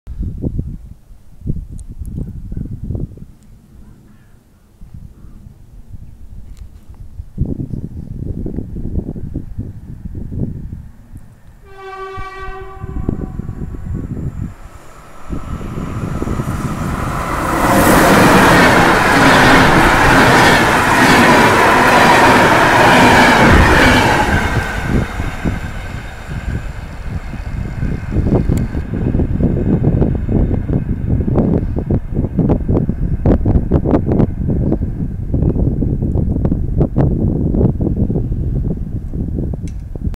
EU44-005 z EC "Berlin-Warszawa-Express"+RP1+Zadymka śnieżna
Piąty Husarz kiedyś w okleinie Niemiec przejeżdża z powrotnym pociągiem EC "Bwe" z Berlina do Warszawy Wschodniej.Film z dedykacją dla kolegi kwiatek243 dzięki za film z kibelków.